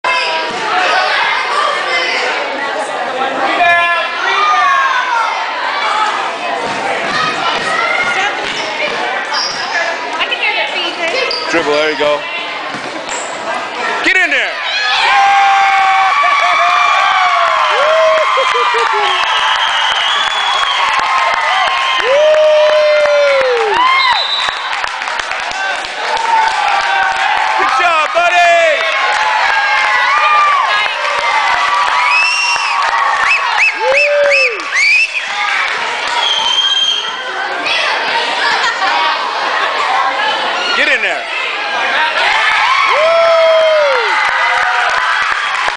JAKE SINKS A BASKET
LAST HOME GAME, AND HE SINKS ONE FROM THE CORNER, #17 , AN' THE CROWD GOES CRAZY.